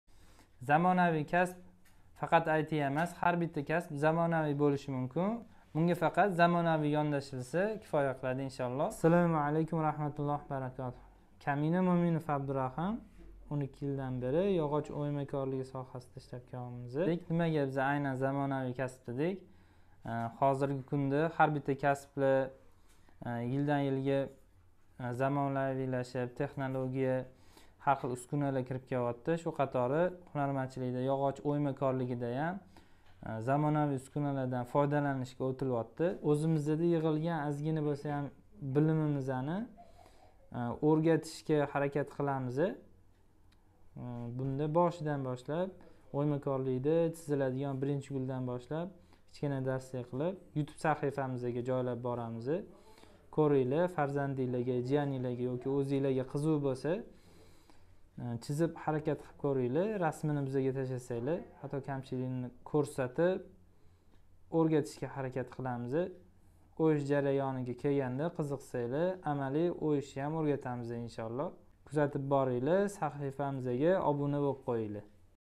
Zamonaviy kasb faqat IT emas!!! Chunki har bir kasb zamonaviy bo’lishi mumkin
Nega aynan sarlavhada unday dedik?

Haqiqatdan ham zamon rivojlangani sari nafaqat IT yo’nalishi balki boshqa yo’nalishlar ham zamonaviylashib borayabdi. Har bir yo’nalish uchun yangidan yangi uskunalar, tehnika va tehnologiyalar kirib kelayabdi, shu jumladan hunarmandchilik sohasida ham yangidan yangi uskunalar kirib kelayabdi.

Shu sabab bosqichma bosqich, qiziqganlar uchun hunarmandchilikni eng oddiy ammo kerakli
bo’lgan darsliklarini ulashmoqchimiz.

Videoni ko’ring va barchasiga o’zingiz ayon bo’lasiz.